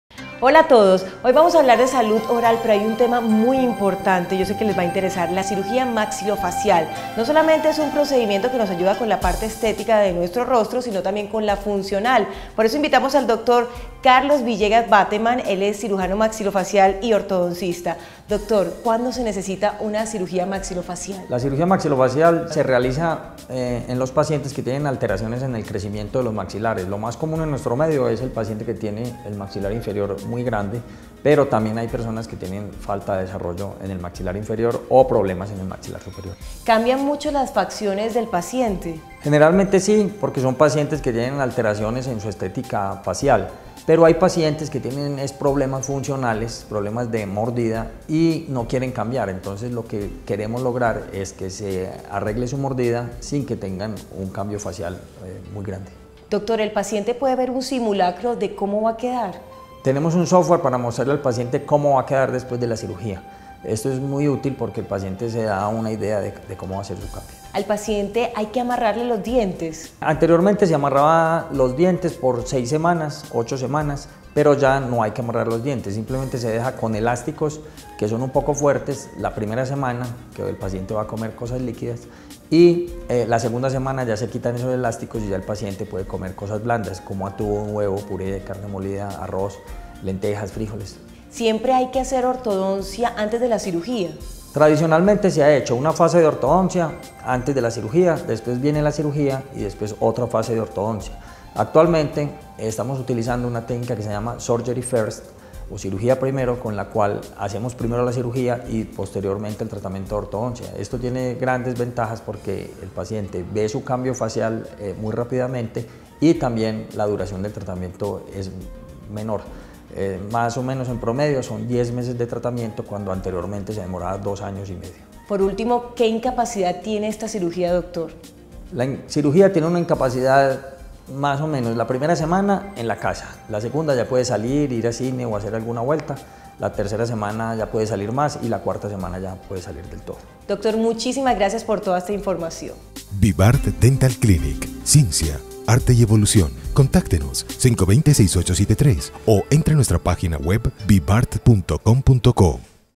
Cirugía Maxilofacial - Vibart Dental Clinic
La cirugía maxilofacial mejora la simetría en el rostro, dando así más estética facial al paciente y una gran mejora en su función masticatoria y en el habla. En Vibart Dental Clinic contamos con tecnología de punta, donde podrás ver una predicción de como quedaras después de la cirugía. Esta misma tecnología nos permite planear digitalmente la cirugía con extrema precisión.